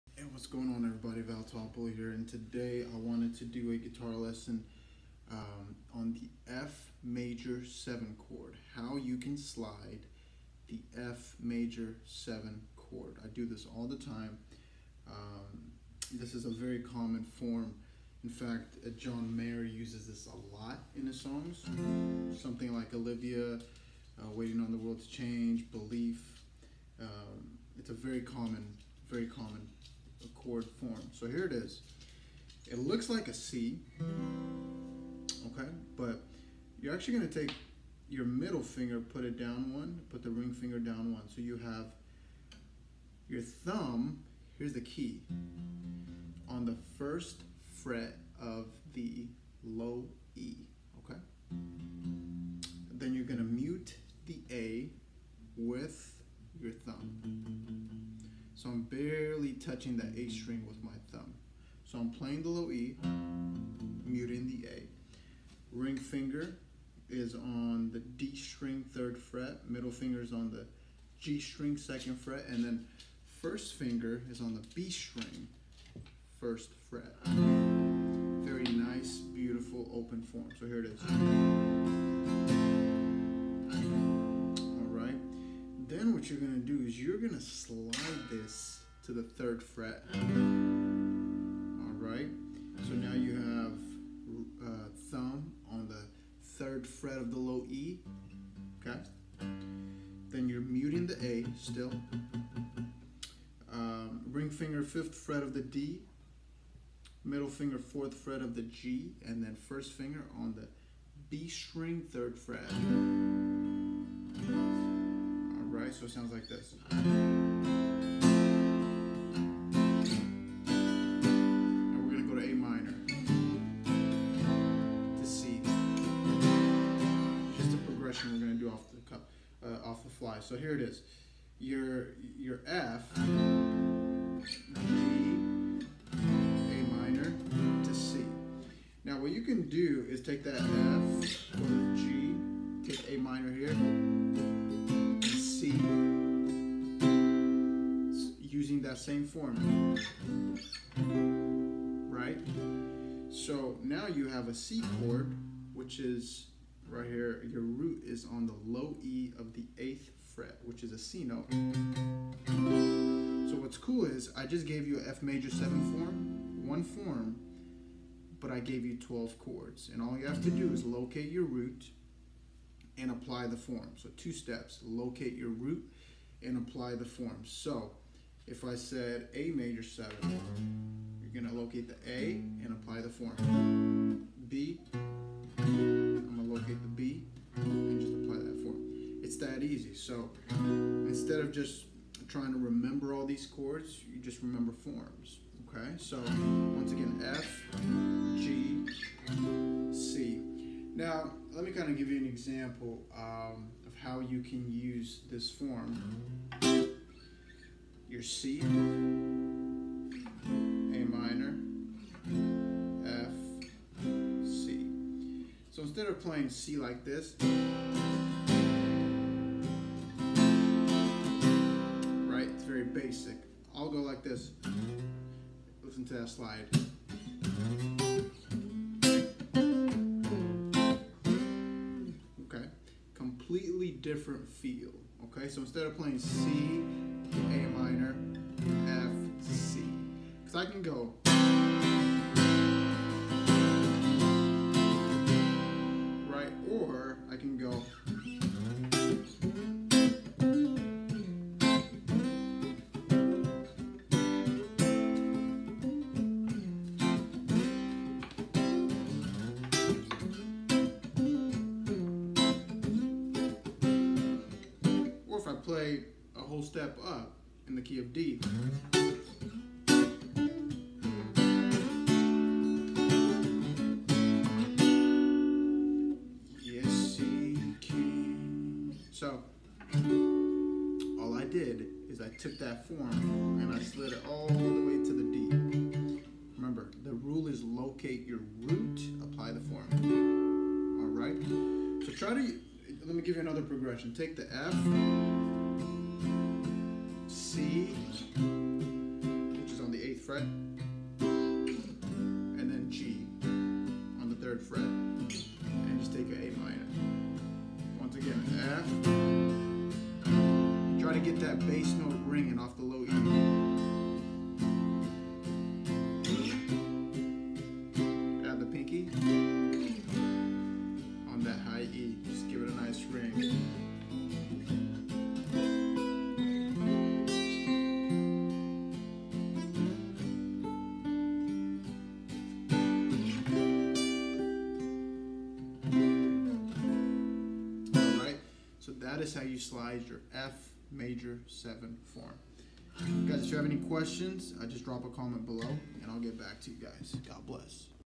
Sliding Your Fmajor7 Chord (COOL TRICK)
In this video, I share how you can slide your fmaj7 chord.